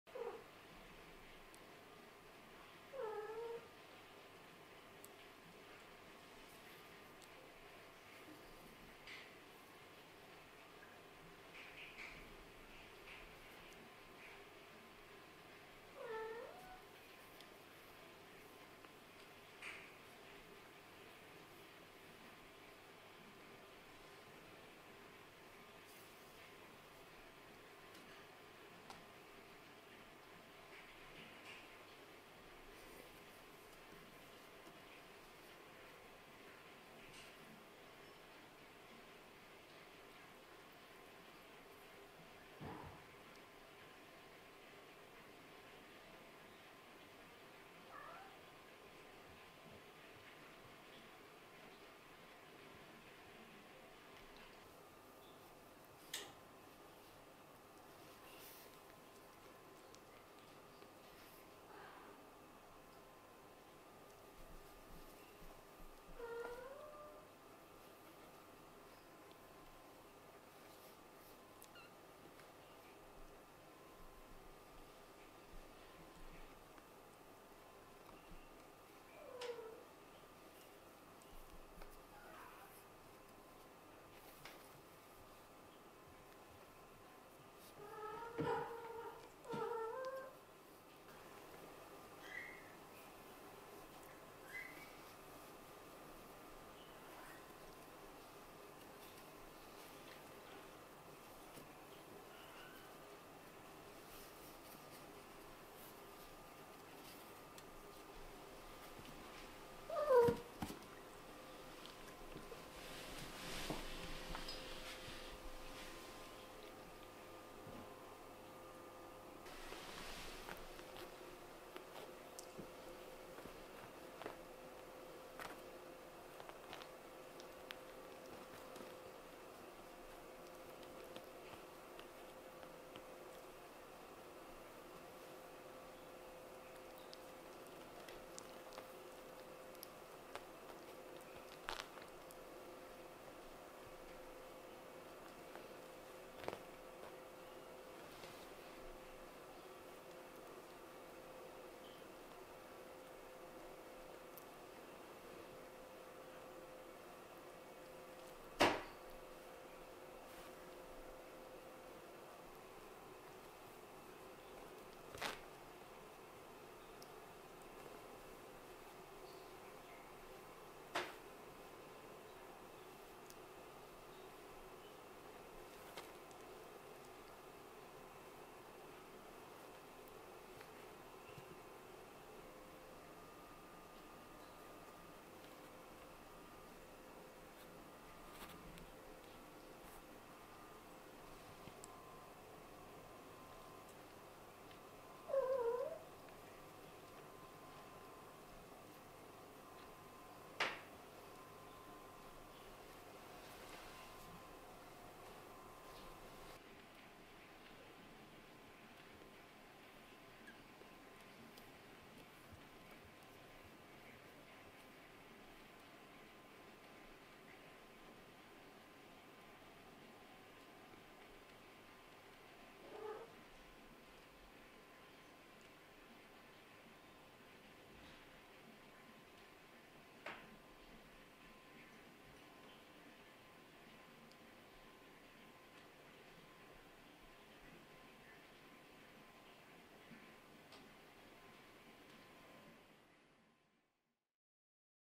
The kittens who come back and stare at me with this expression are too precious.
A kitten who comes back from work and shows affection with this expression.
Although he still wants to sleep, he starts playing to show his owner that he is doing well.
But after all, the sleepy kittens are too cute to gather and sleep together again.

●Lulu
Mother/Scottish Fold/Blue Tabby
● Leo
Father / british shorthair / shell silver shaded
●Mie
eldest daughter / blue tabby / short hair
●Coney
second daughter / blue tabby & white / long hair
● Latte
eldest son / white & brown / short hair
● Tete
second son / white & blue / short hair
● Coo
third son / blue & white / short hair

Click here if you want to see a kitten that is condensed with cuteness ↓
✅ Kitten Class